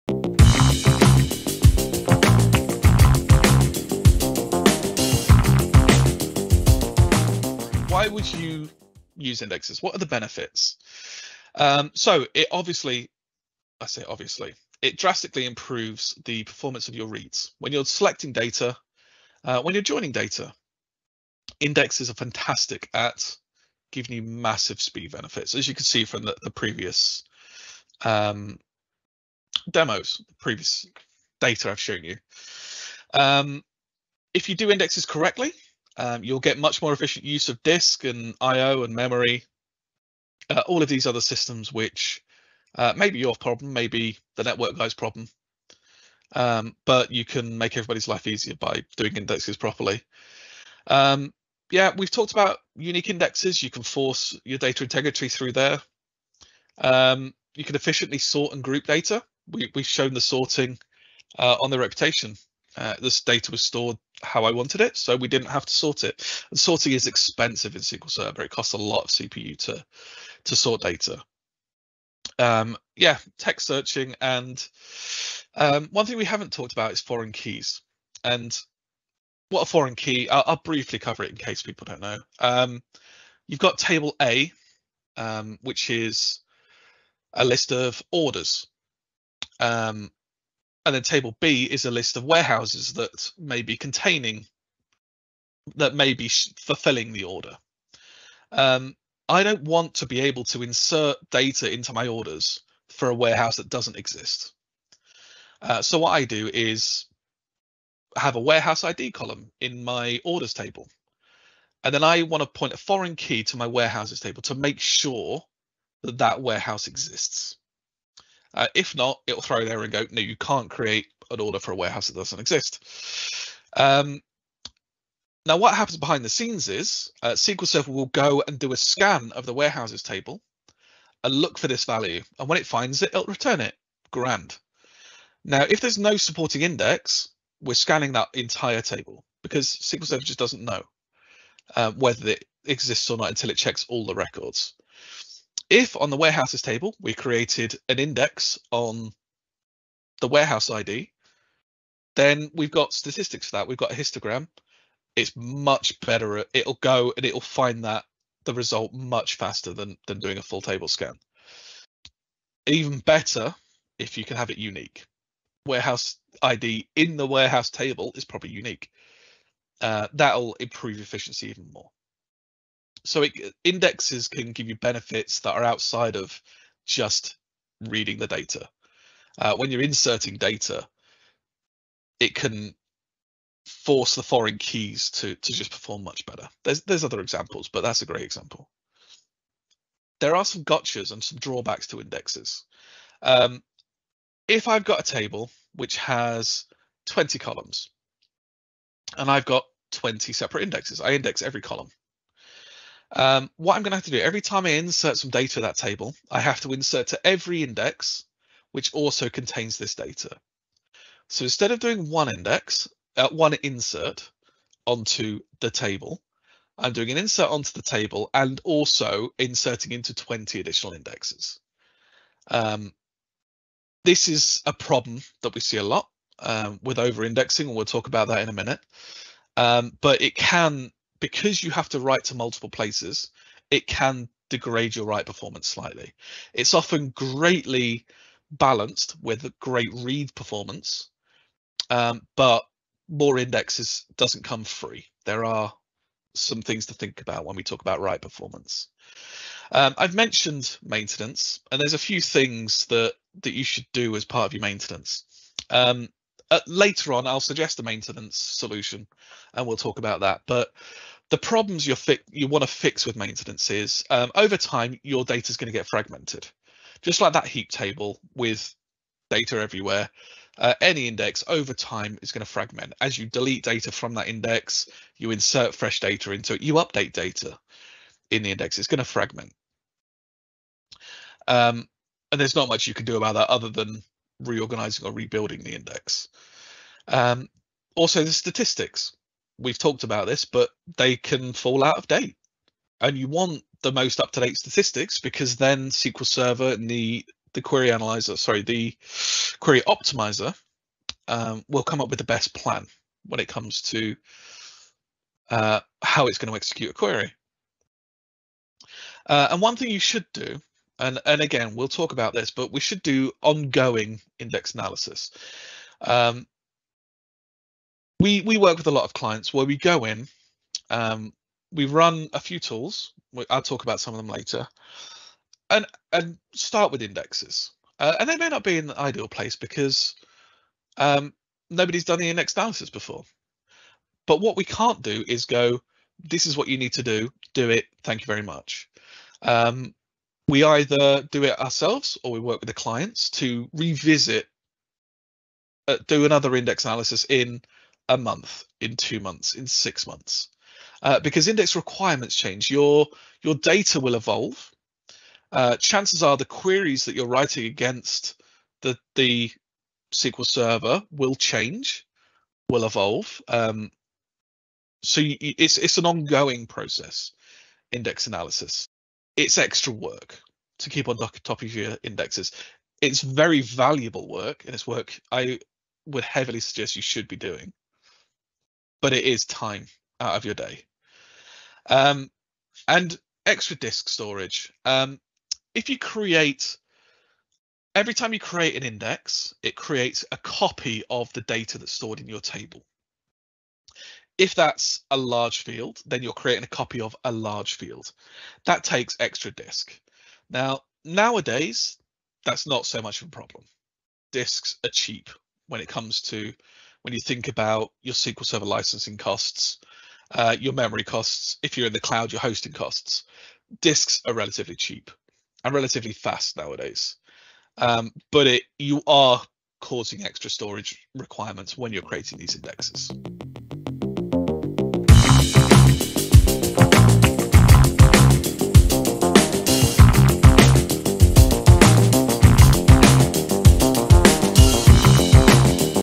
SQL SERVER 7 min EXPLAINER- Index Advantages & Disadvantages with Rich Benner
SQL Solutions Group Senior DBA Rich Benner explains the advantages and disadvantages associated with Indexing. Excerpt from the recent webinar "Indexing SQL Server"